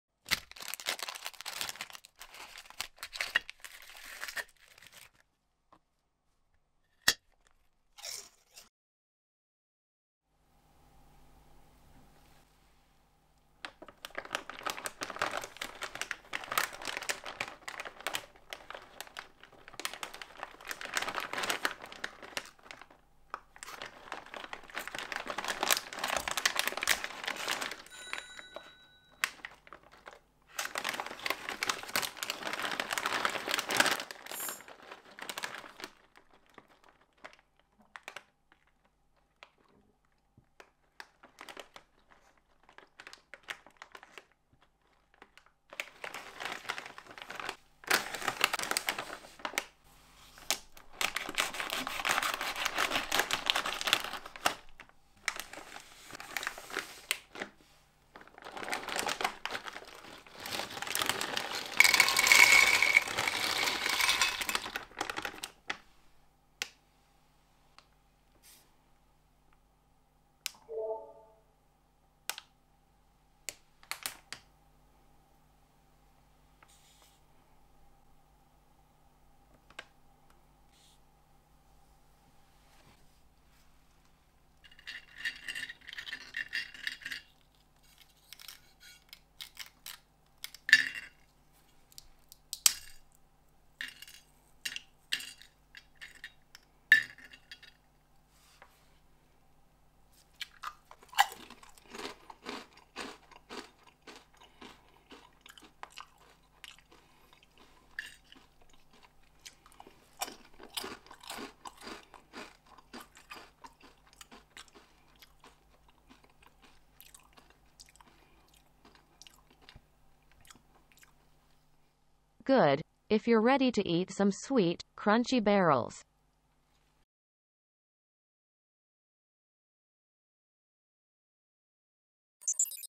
Samyang Chang Gu Honey Dipped Snack (115g) 🍯🇰🇷 [Judgment Day: 0101]
Snack unboxing, day 101
Today's subject: Samyang Chang Gu Honey Dipped Snack (115g) 🍯🇰🇷
Verdict: 👍 (good)
I'll be snack...